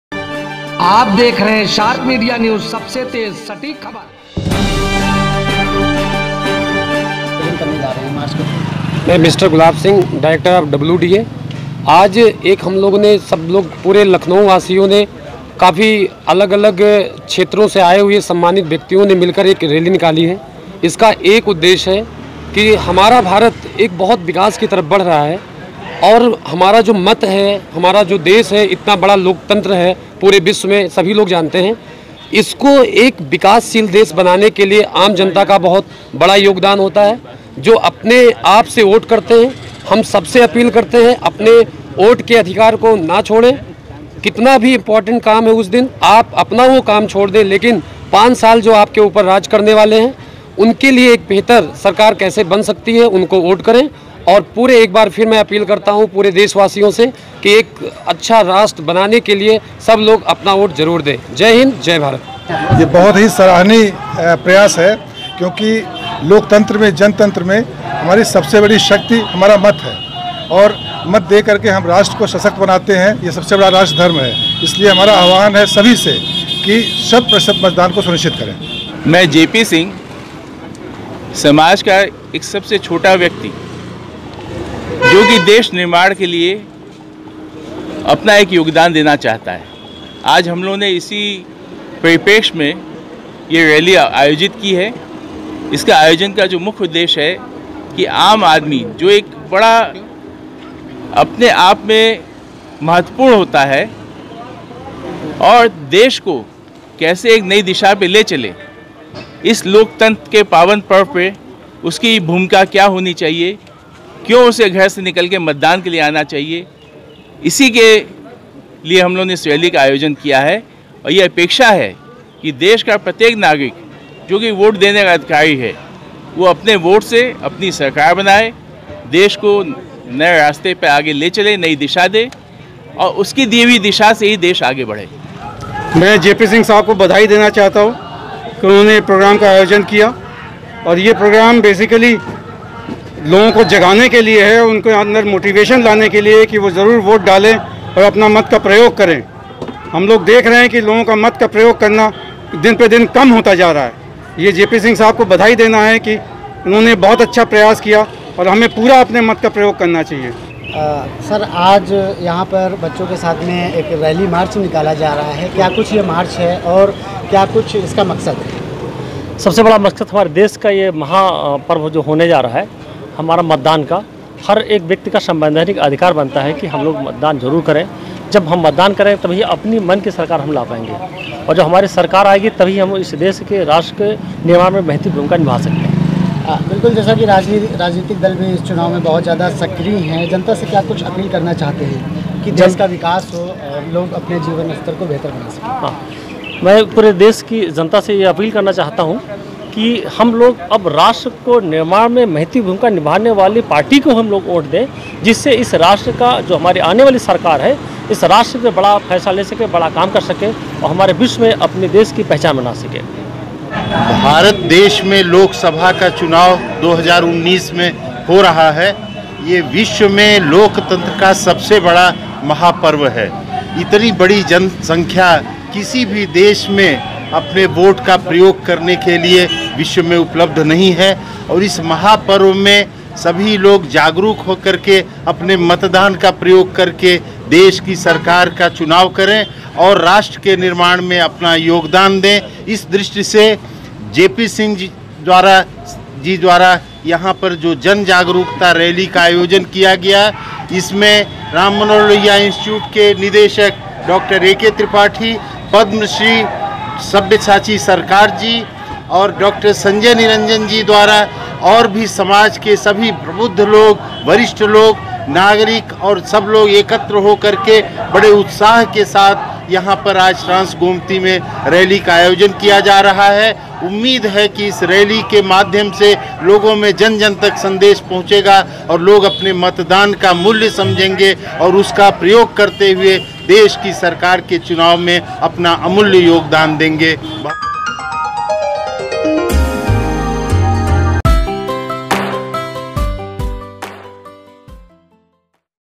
रैली निकाल कर मतदाताओं को किया गया जागरूक
SharpMedia लखनऊ:वैरिएर्स डिफेंस एकेडमी व महानगर समाज कल्याण एशोसिएशन की तरफ सार्वजनिक मतदान जागरूकता रैली निकाली गयी जिसमे लखनऊ के तमाम संवेदनशील हस्तयो ने बढ़ चढ़कर हिस्सा लिया वैरिएर्स डिफेंस एकेडमी के सैकड़ो बच्चों के साथ रैली राजधानी के अलग अलग हिस्सों में जा कर लोगो को मतदान के लिए जागरूक किया तो वही डिफेंस एकेडमी के युवा डायरेक्टर गुलाब सिंह ने पूरी रैली का नेतृत्व किया अपनी बात में गुलाब सिंह ने बताया रैली आयोजन का मकशद समाज मे रह रहे हर तबके को मतदान के लिए जागरूक करना है ।
रैली आयोजन में SR ग्रुप के चेयरमैन पवन सिंह चौहान,महानगर समाज कल्याण एशोसिएशन के अध्य्क्ष जे पी सिंह,केजीएमयू कुलपति प्रो एम एल बी भट्ट, पदम् श्री एस एस सरकार, राम मनोहर लोहिया निदेशक ऐ के त्रिपाठी व मिडलैंड हॉस्पिटल के संस्थापक डॉ वी पी सिंह सहित तमाम संवेदनशील हस्तियों ने रैली में शामिल होकर मतदान जाकरूकता अभियान को सफल बनाया । साथ ही सब ने आम नागरिक के मतदान अधिकार को बहुत आवश्यक बताया और एक मजबूत राष्ट्र व देश की तरक्की के लिए सभी से मतदान के लिए अपील की ।